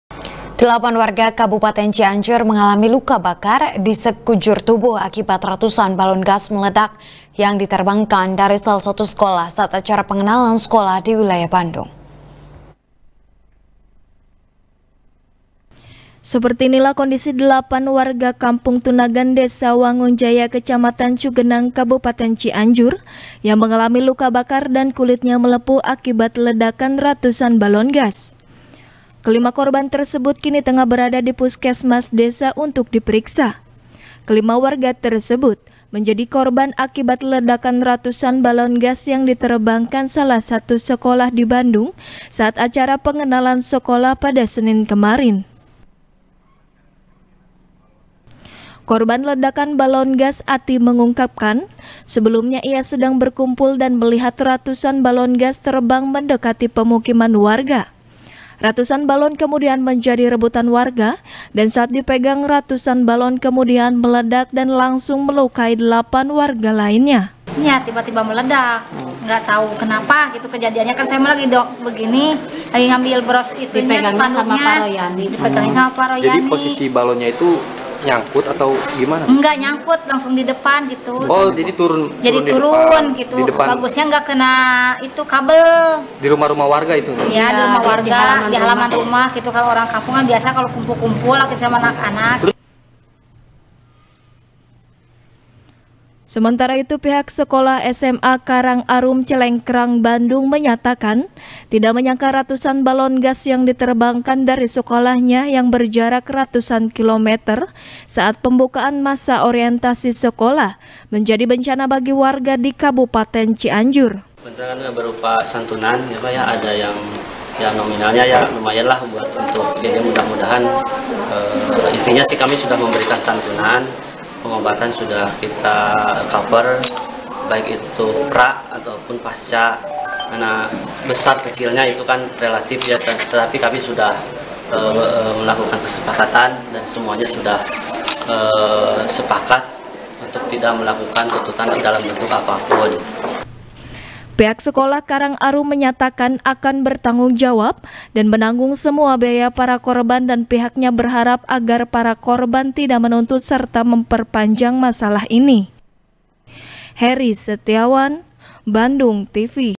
Delapan Orang Warga Menjadi Korban Balon Gas MPLS | SBR | BANDUNGTV
SBR | BANDUNGTV | NIKEN PRATIWI | Delapan Orang Warga Kabupaten Cianjur,Mengalami Luka Bakar Di Sekujur Tubuh,Akibat Ratusan Balon Gas Meledak,Yang Diterbangkan Dari Salah Satu Sekolah Saat Acara Pengenalan Sekolah Di Wilayah Bandung .